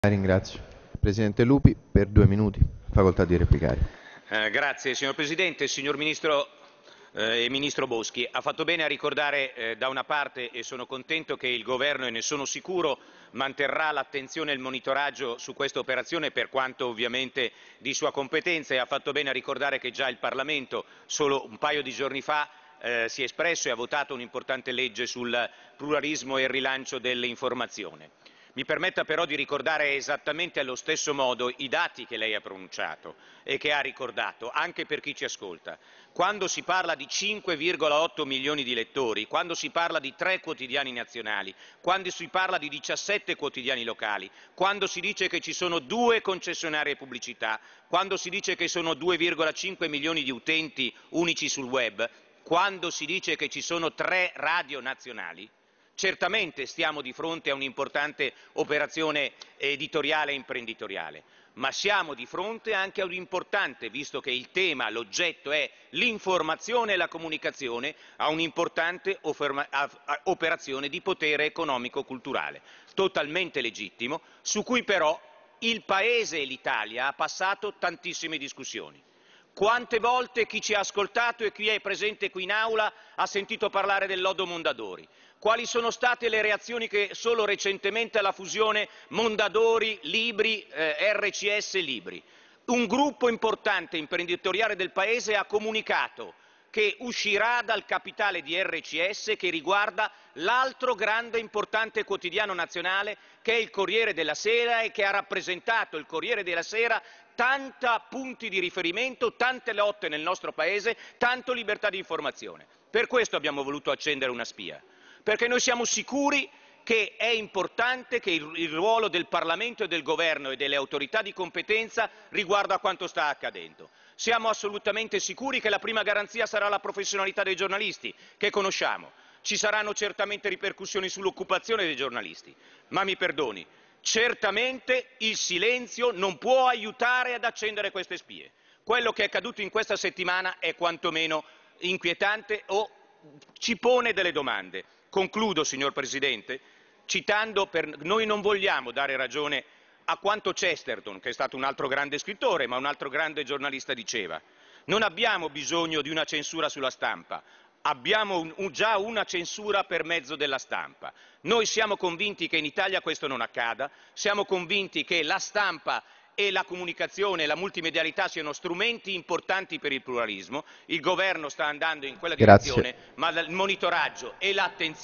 Lupi interroga ministro Boschi su fusione Itedi-Espresso
Interrogazione di Maurizio Lupi al ministro per i Rapporti con il Parlamento, Maria Elena Boschi,